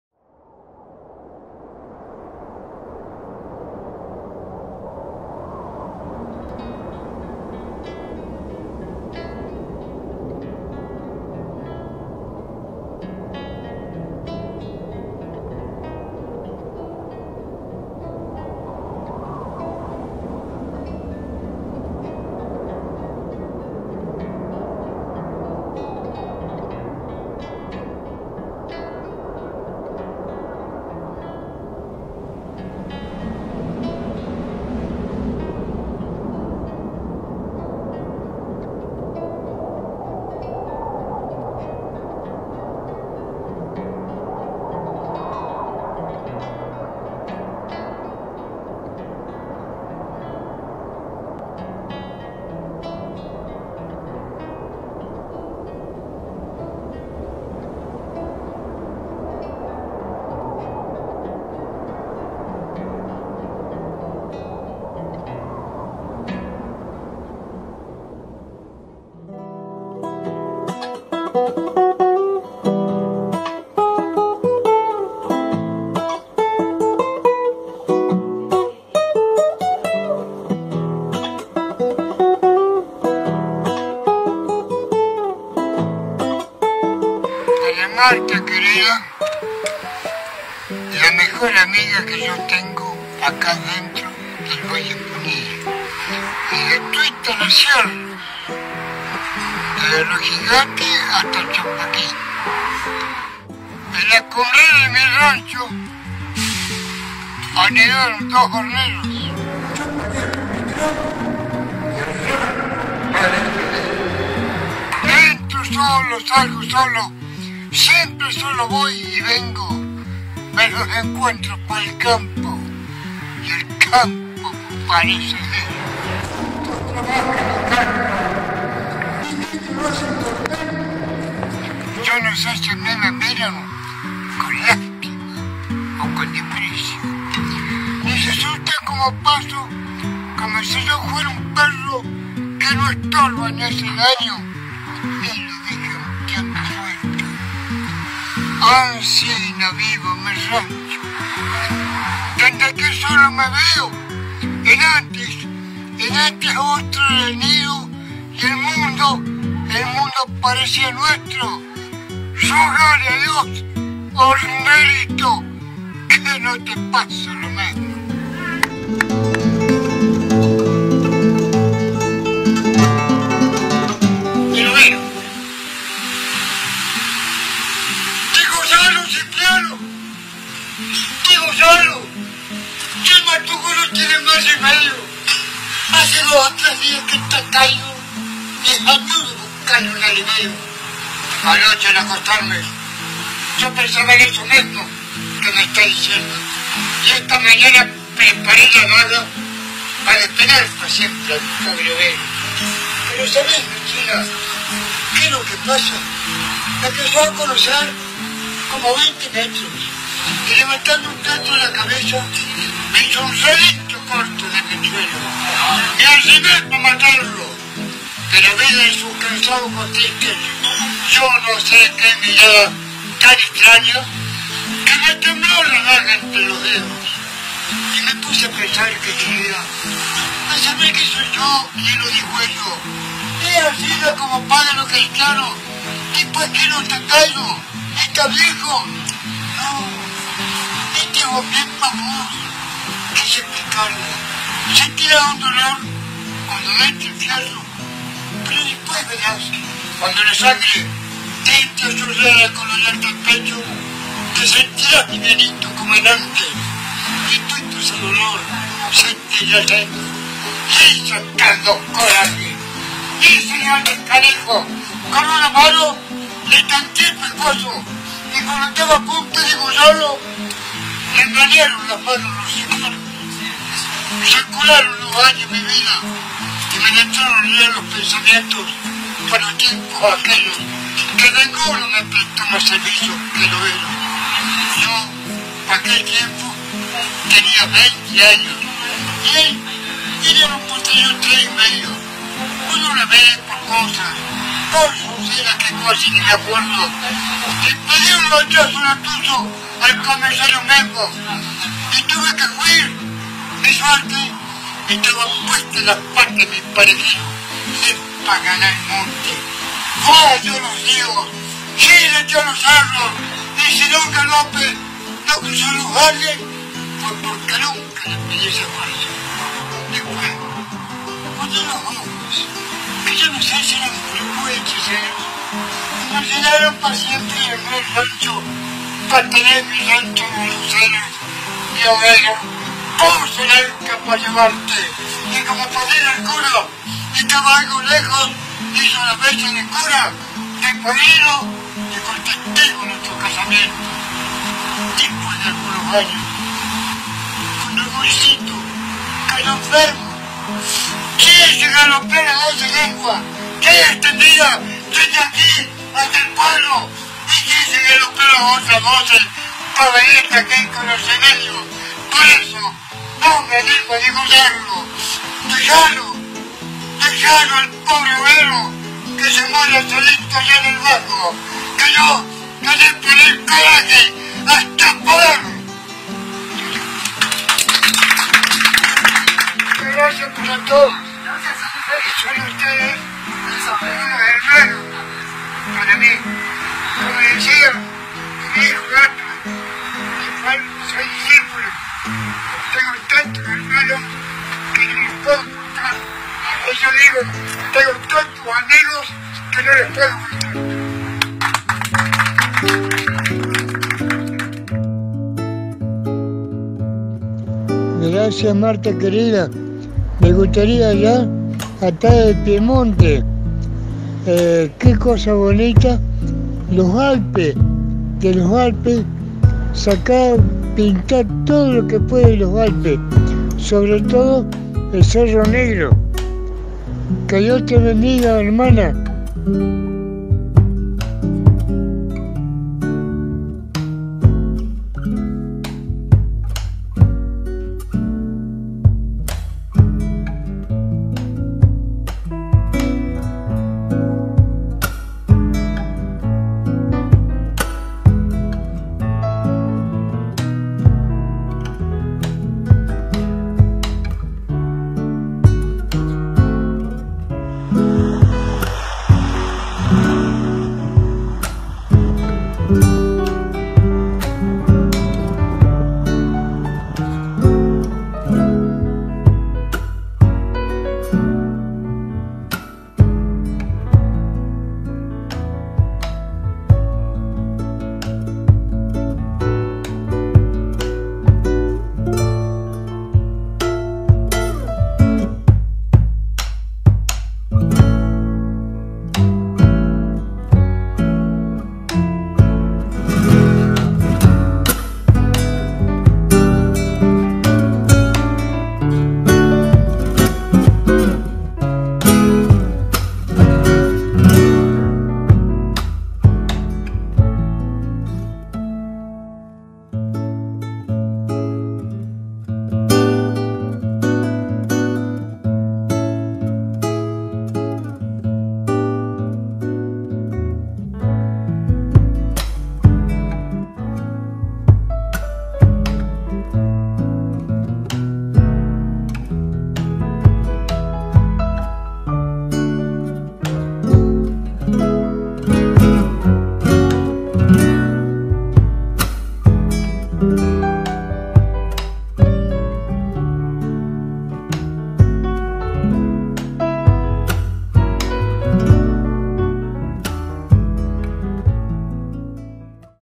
José Luis Pearson ''El Overo"
José Luis Pearson, "El Overo" , Nació en Pergamino, Provincia de Buenos Aires el 6 de marzo de 1935.
Recitador Criollo.
Lo conocí en 2009 y cultivamos una hermosa amistad.
La montaña, la geografía, los caminos de las sierras, eran algunos de los motivos de nuestras charlas.
Aquí les dejo mi Homenaje a este ser de luz , que dejó este mundo el 16 de enero de 2018.